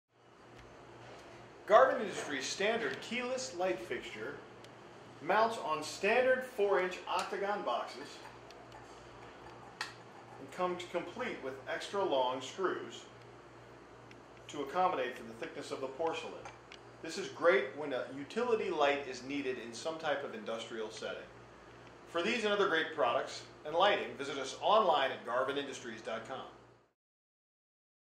Round Lamp Holder: 4" Round Porcelain Lamp Holder for 4" Octagon Box & Raised Ring PF-50
- 4" round porcelain lamp holders are mounted on octagon boxes and raised rings when permanent convenience lights are needed. Garvin Industries' lamp holders allow the installer to economically add an exposed light source to applications where covered lamps are not required. Lamp holders have a quick and easy installation in many locations.